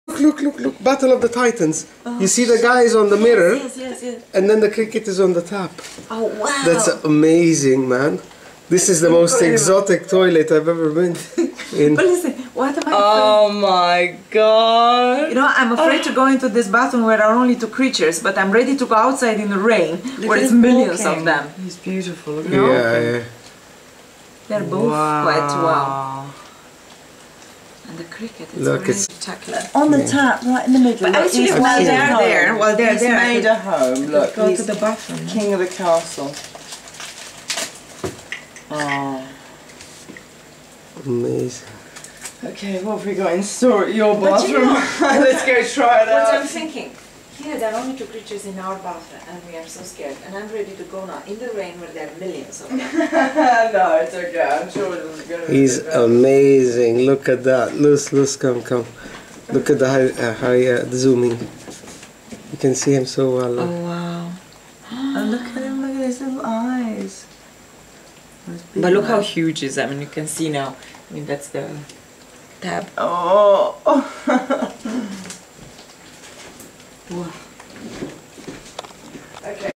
Giant Beetle and Grasshopper lay seige
In Siladen, an island off the Indonesian coast of Sulaweisi, a large beetle and even larger grasshopper decide to call our bathroom home for the night...